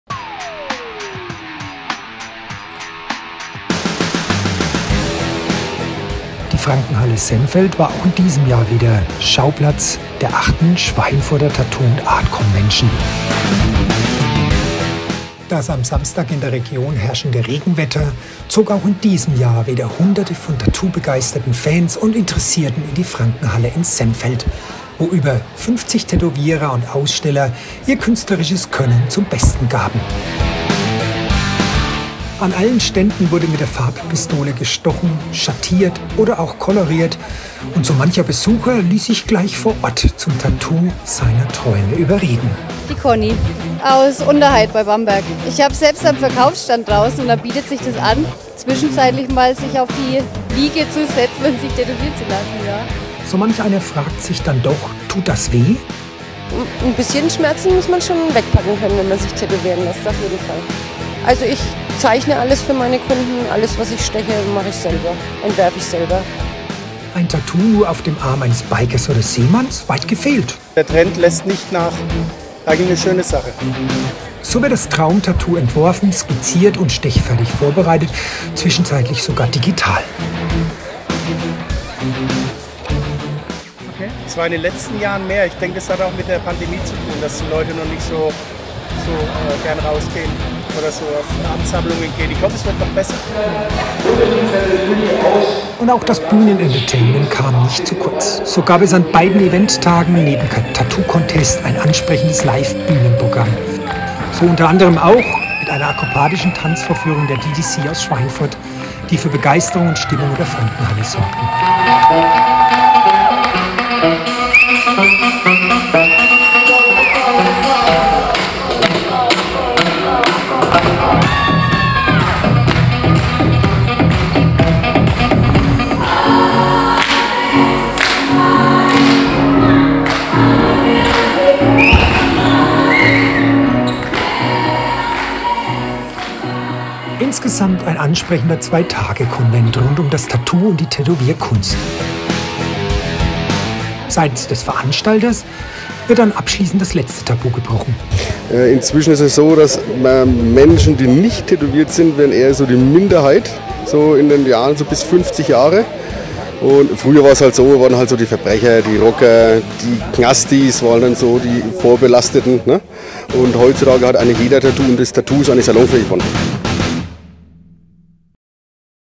Tattoo Convention in Sennfeld 2022
SENNFELD - Am Samstag und Sonntag war in der Frankenhalle in Sennfeld die 8. Tattoo & Art Convention. Rüdiger Wolf war für SW-N-TV vor Ort und hat sich dort umgeschaut. Mehr im Filmbericht.